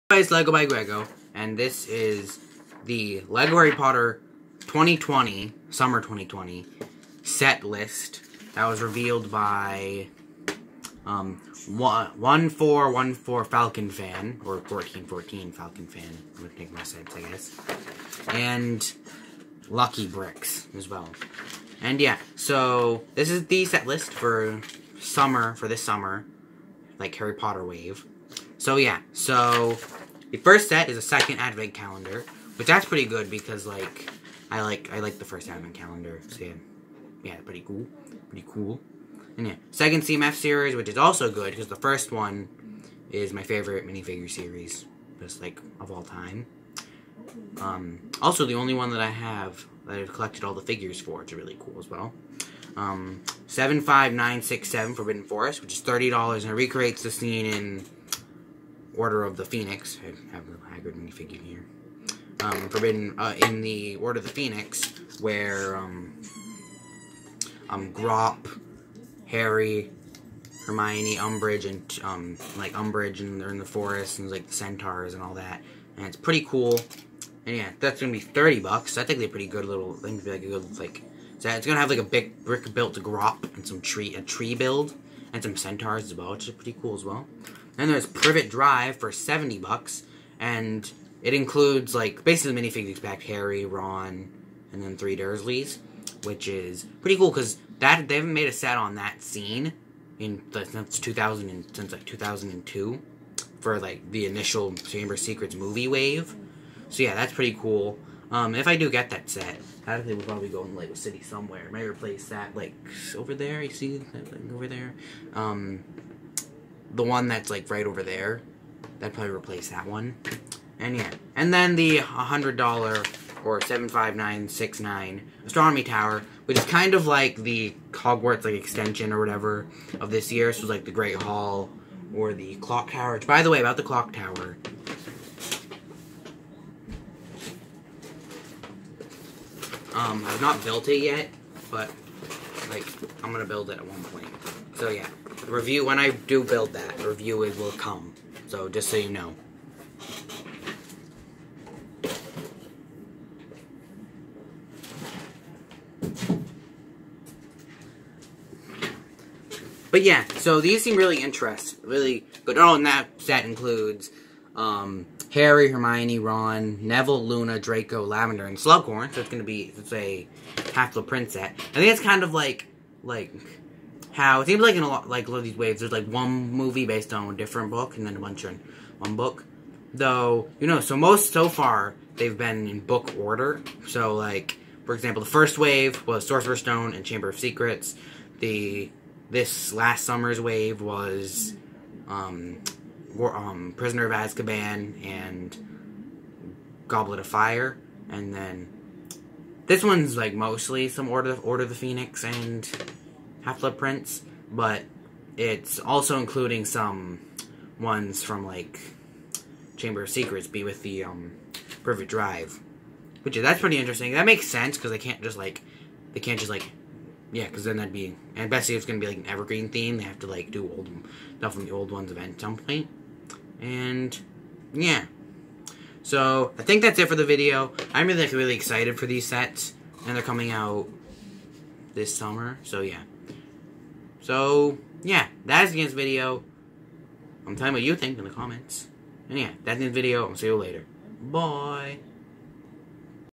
Lego Harry Potter Summer 2020 Set List Thoughts
These seem really cool
Info from 1414 Falcon Fan and Lucky Bricks